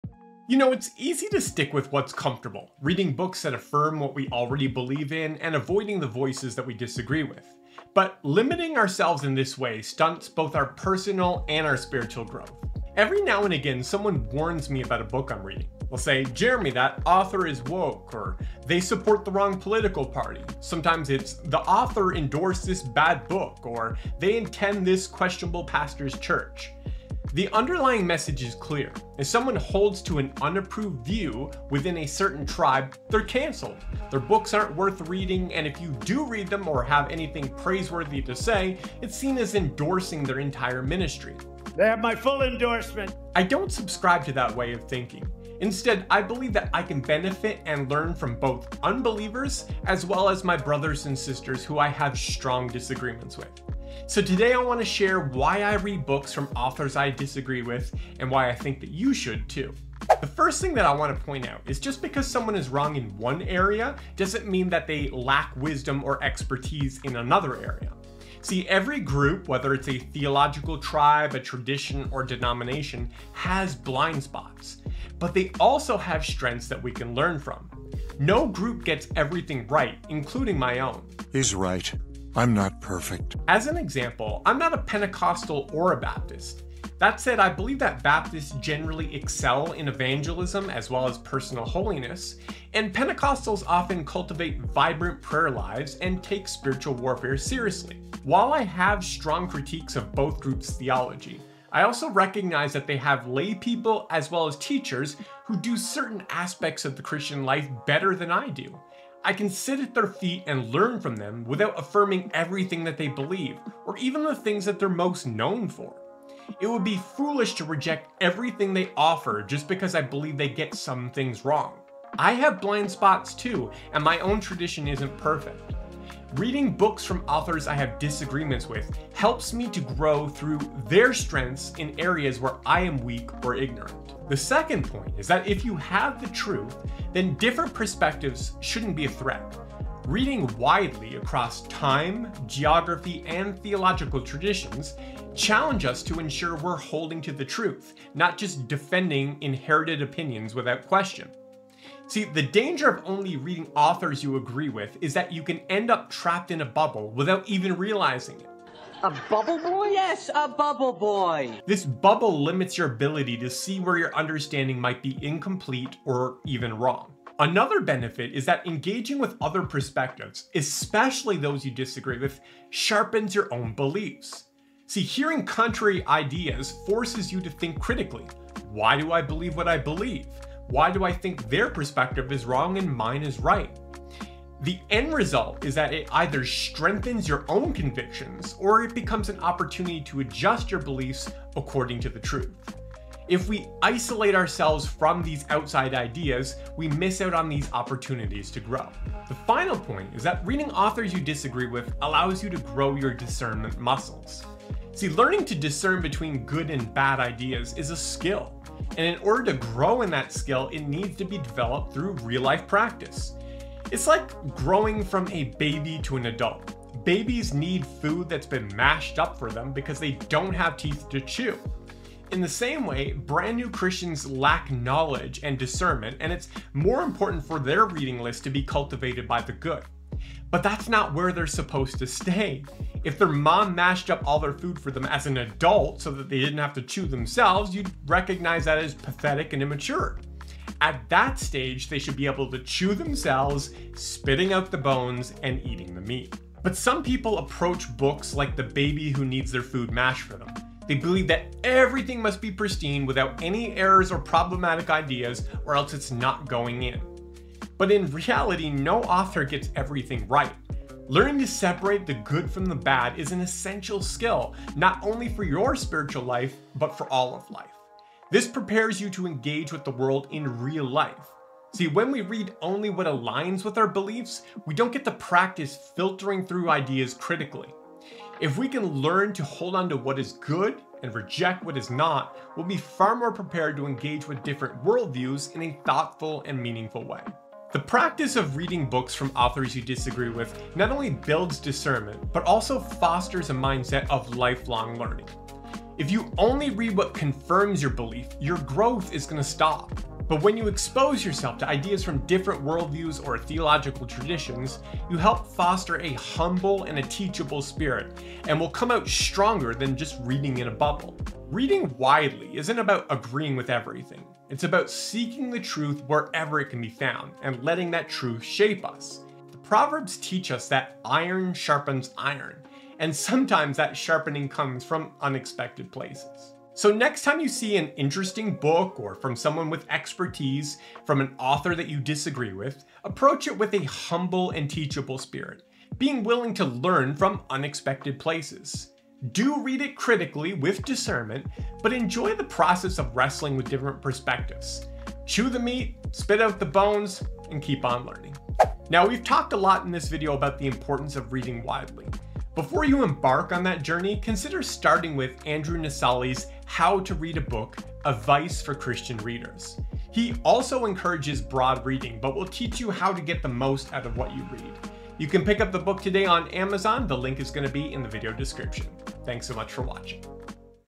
Why You Should Read Books You Disagree With
In this video, I share why it’s essential to read books from authors you disagree with and how doing so fosters personal growth, strengthens your beliefs, and builds discernment.

Limiting yourself to voices you agree with can trap you in a bubble, stifling critical thinking and lifelong learning. Instead, I encourage you to engage with different perspectives, chewing the meat and spitting out the bones, as you seek truth wherever it can be found.

Let’s explore why embracing diverse ideas can sharpen your faith and make you stronger.

📚 Book Recommendation
How to Read a Book: Advice for Christian Readers by Andrew David Naselli

Get it on Kindle or Hardcover from Amazon today.

📜 Video Chapters
0:00 - Introduction
0:59 - Everyone has Strengths
2:14 - Avoid the Bubble Life
3:19 - Build Discernment Muscles
4:47 - Conclusion: Fostering Lifelong Learning
5:48 - Book Recommendation